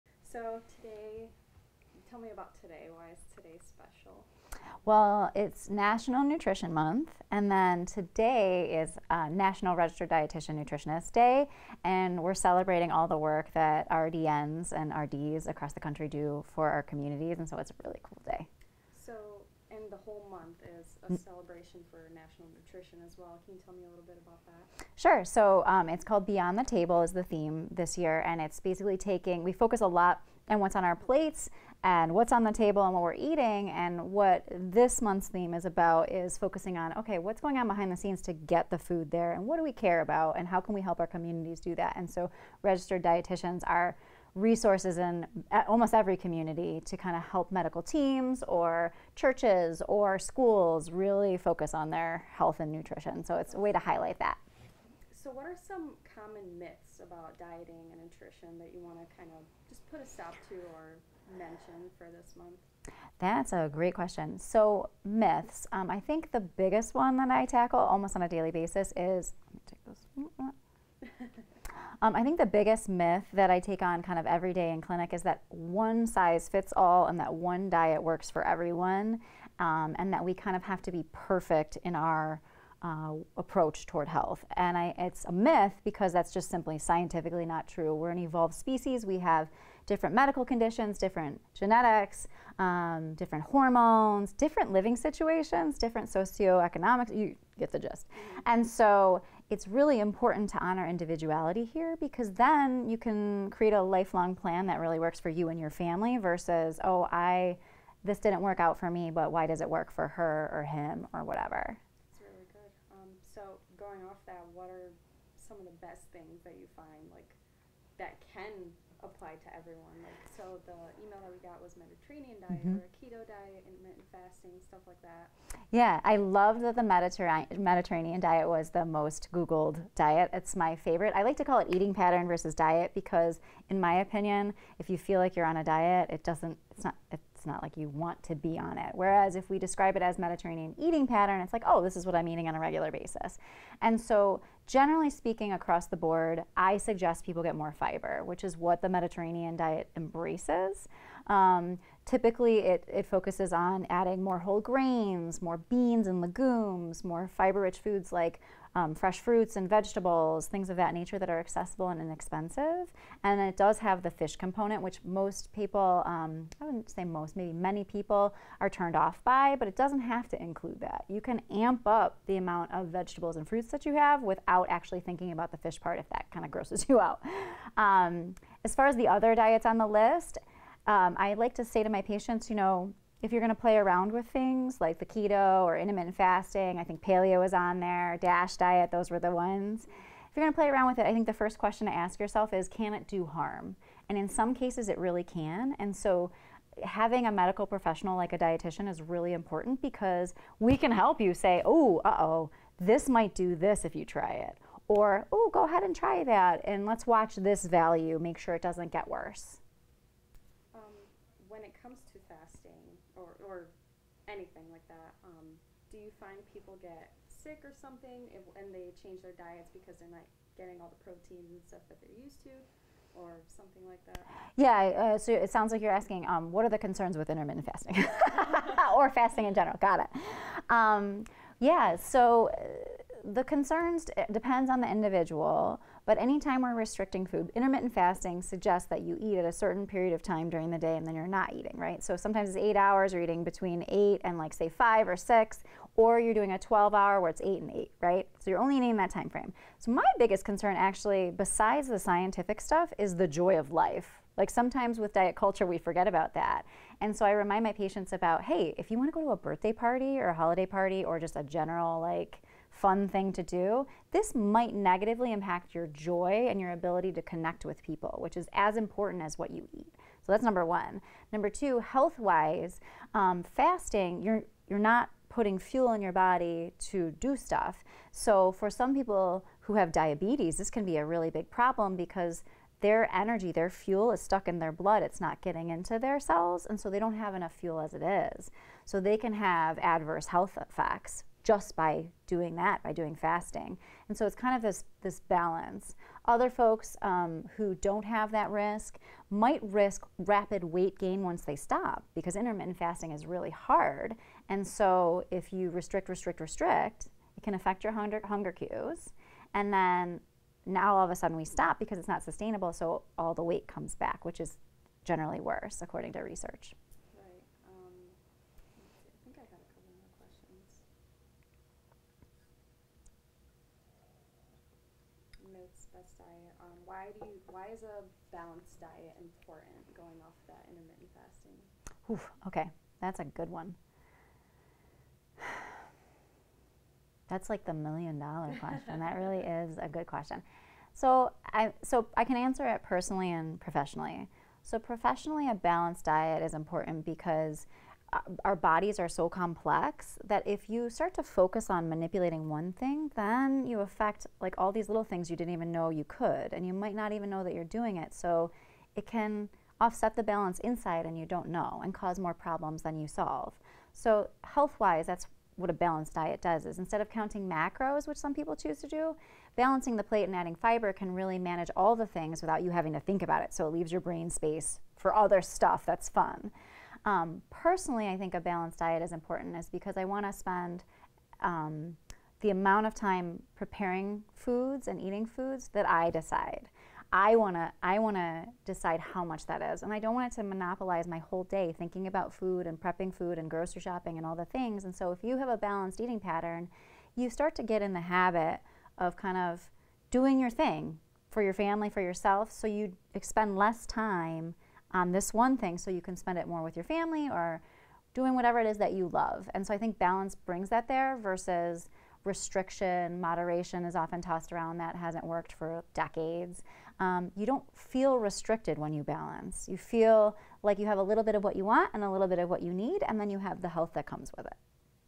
National Nutrition Month
A local registered dietitian talks about the importance of National Nutrition Month and offers her take on the most-searched-for diets.